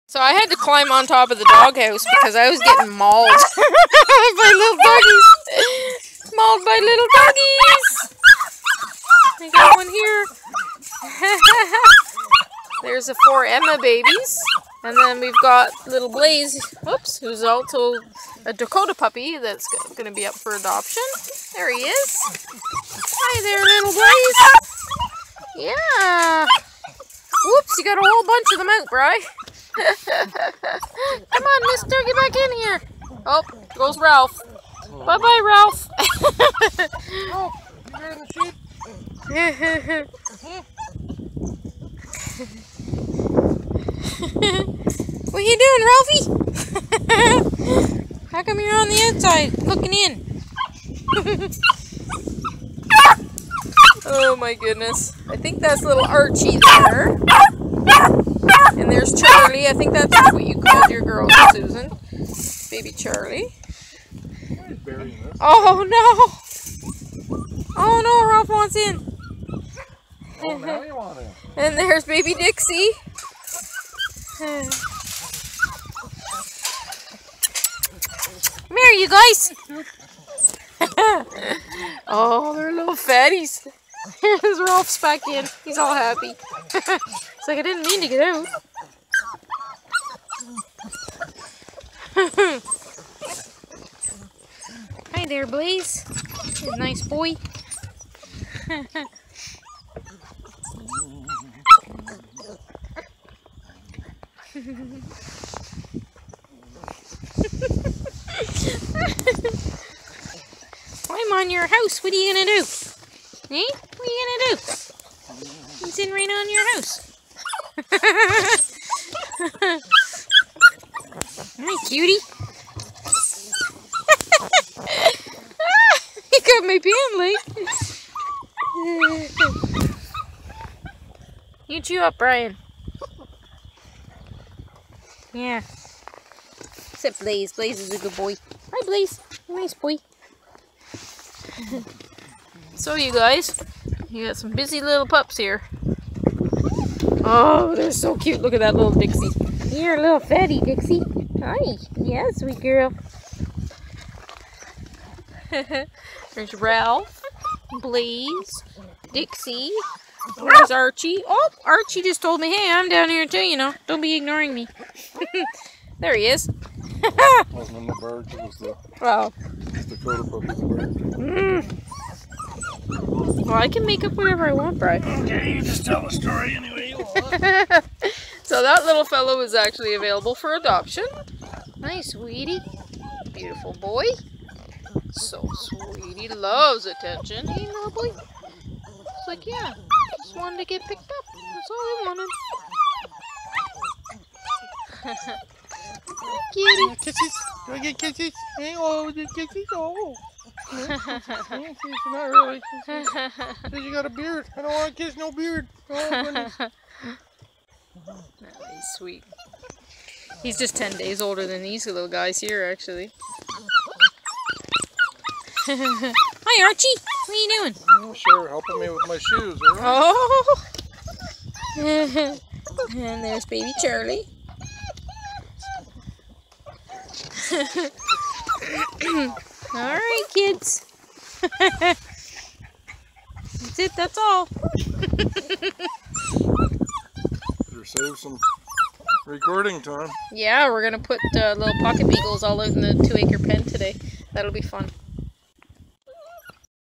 7.5 week LEMON BEAGLE PUPPIES
These pups are heading home this week!